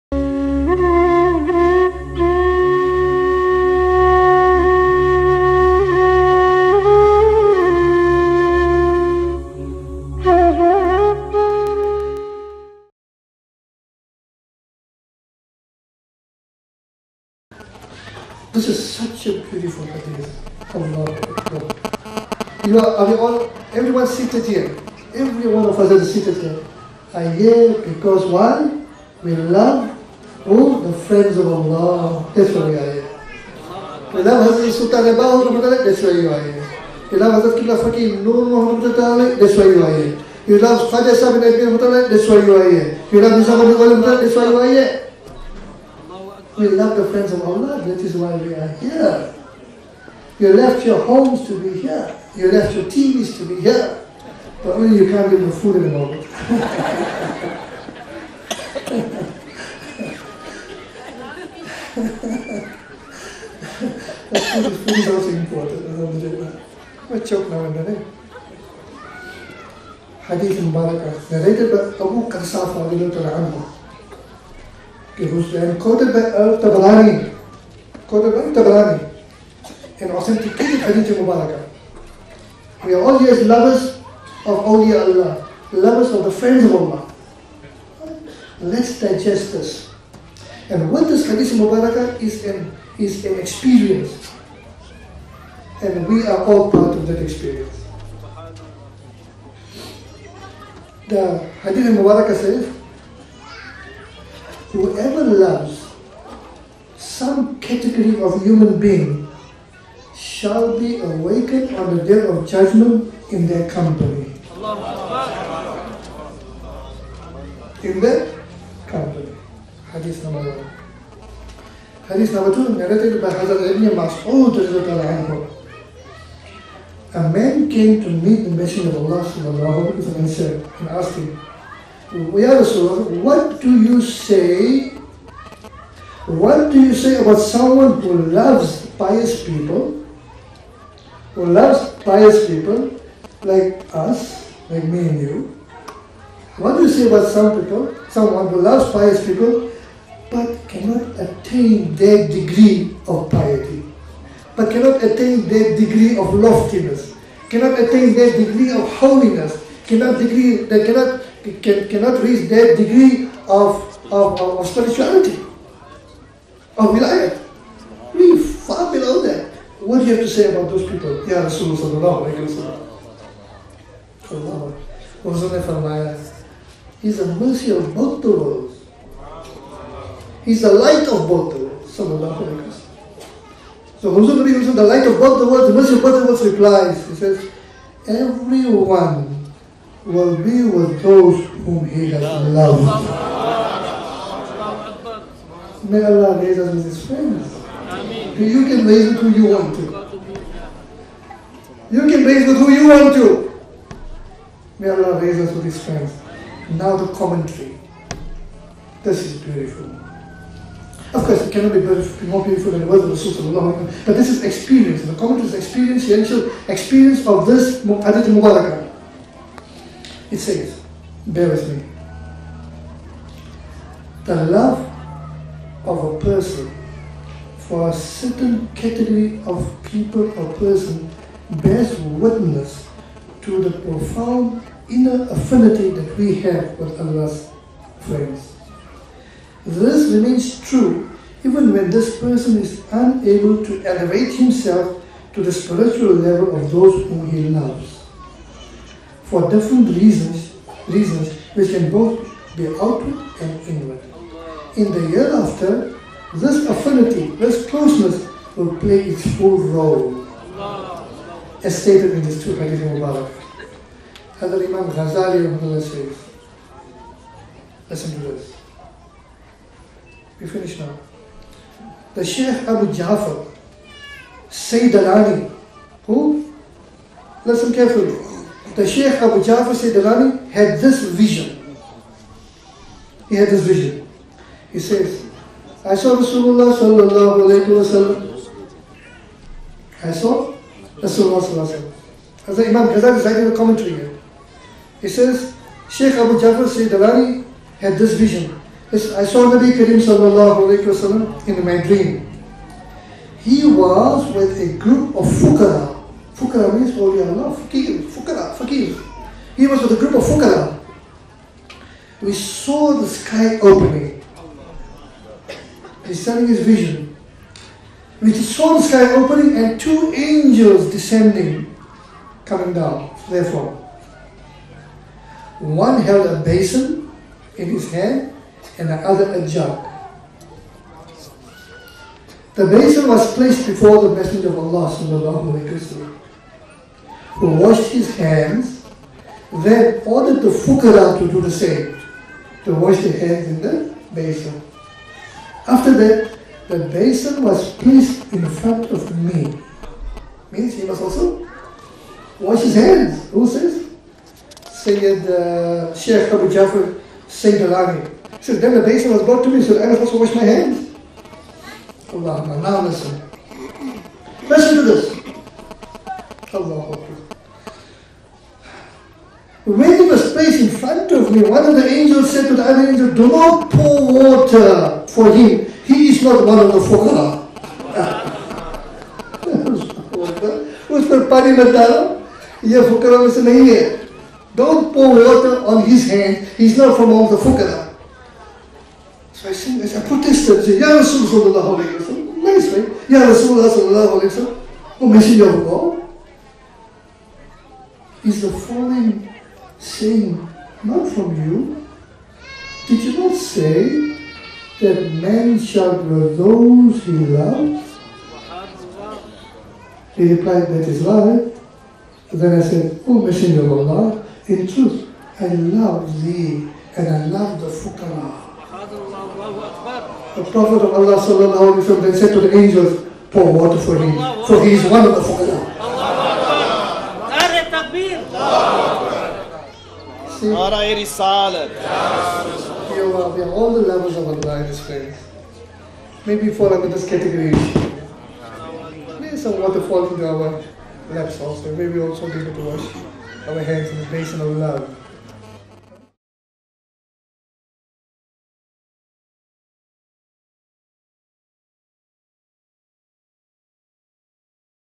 Love for the Awliya: What Rasulullah(s) said about it
A short video about what Rasulullah(s) said about those who love the Awliya.